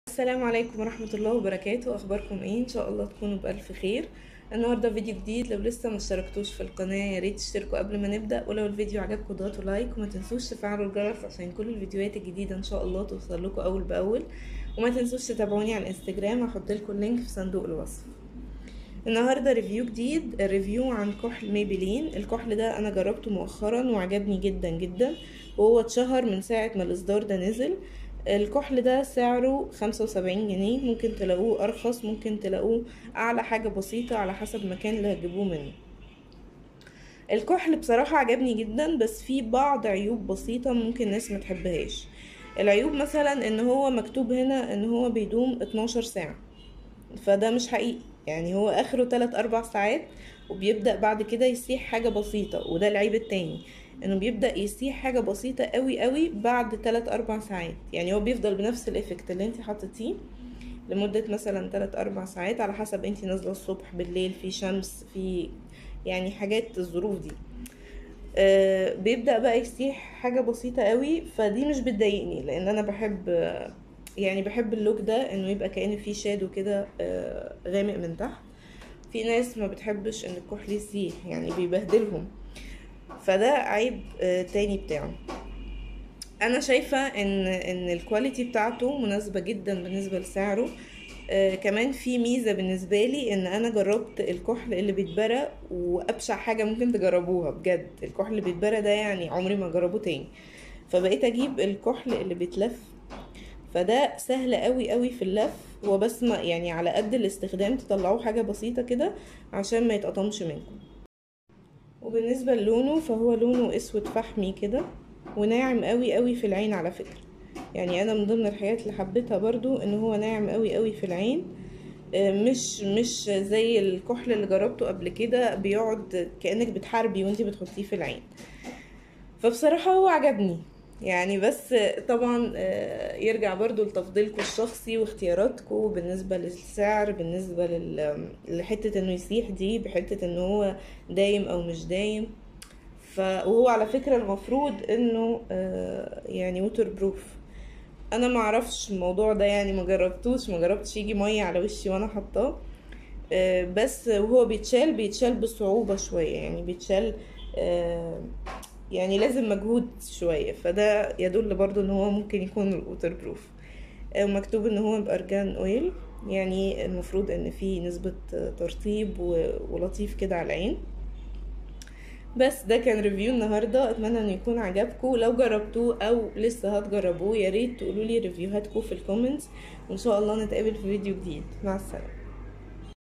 ريفيو عن كحل ميبلين الاصفر - سعره مميزاته و عيوبه
حساب انستجرام ❤️